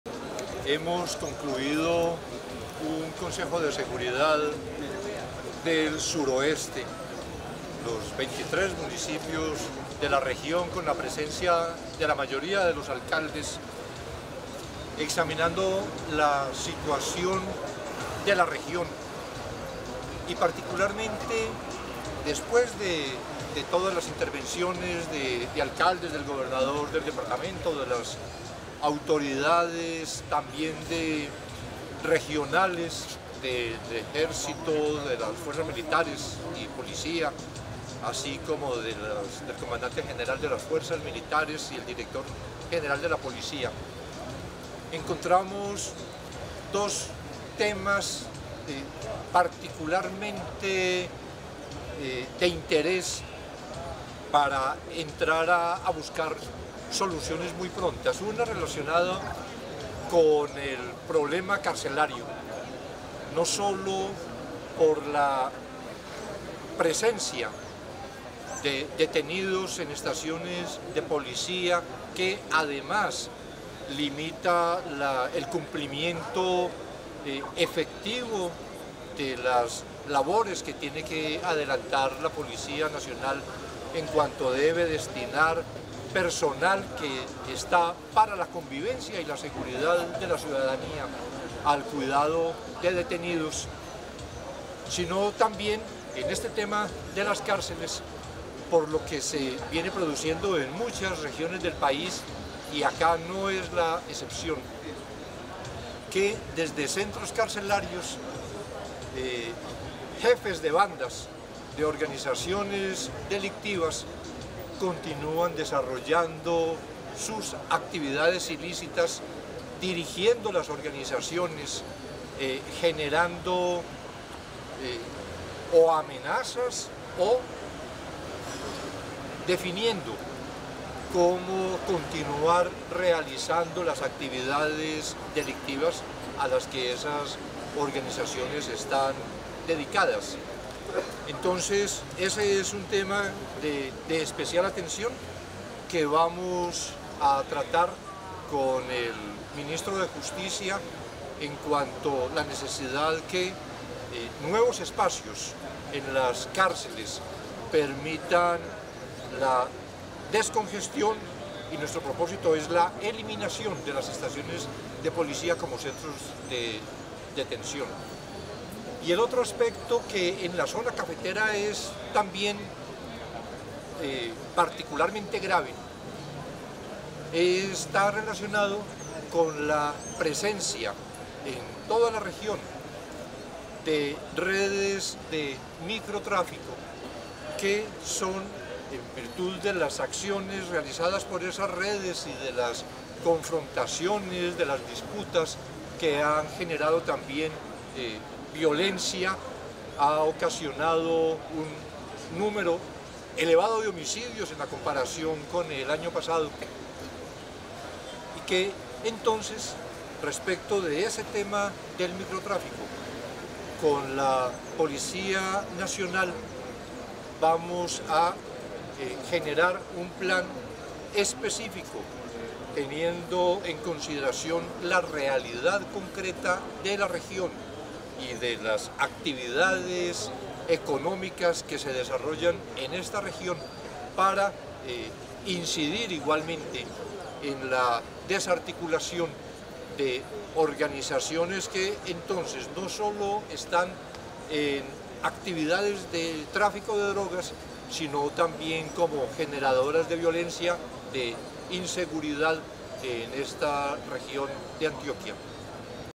Declaraciones Mindefensa, Iván Velásquez, luego de Consejo de Seguridad en Ciudad Bolívar, Antioquia
Luego de Consejo de Seguridad en Ciudad Bolívar, Antioquia, el ministro Iván Velásquez anunció:

✅Plan específico contra el microtráfico en el Suroeste Antioqueño

✅Trabajo articulado con MinJusticia para eliminar estaciones de policía como centros de detención en la región